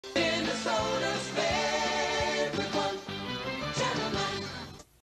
KMSP (Independent, Now Fox) Station ID 1993 "Minnesota's Favorite One"
This is KMSP's station ID from 1993
KMSP is located in Minneapolis-St. Paul, MN